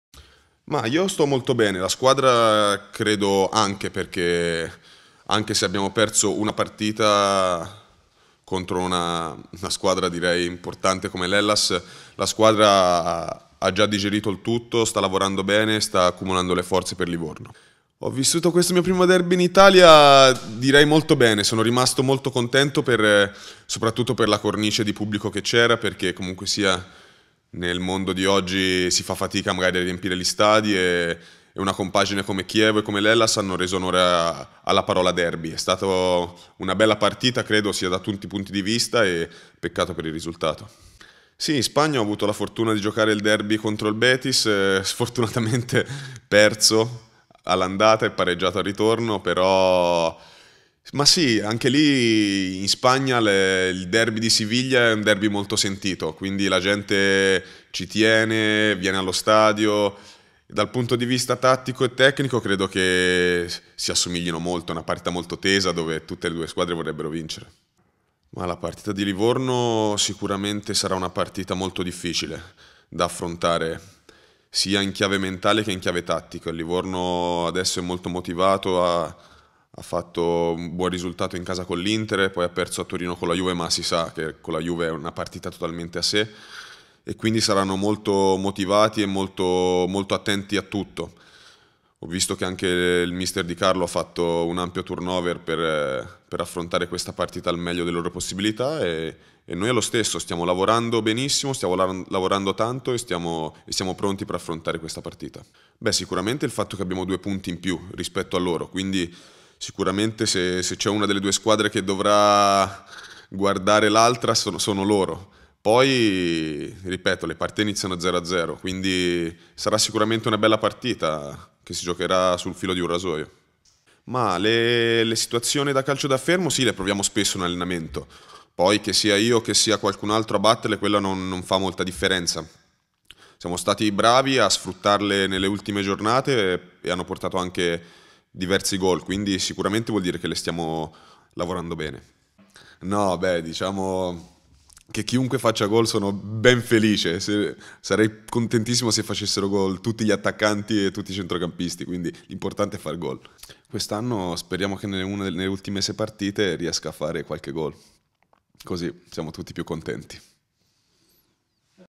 Tiberio Guarente e la corsa salvezza
Il centrocampista gialloblù Tiberio Guarente ai microfoni di Chievo Tv: "La matematica ti frega. I conti non si fanno con le ultime sei partite che restano ma con i palloni che conquisti in campo".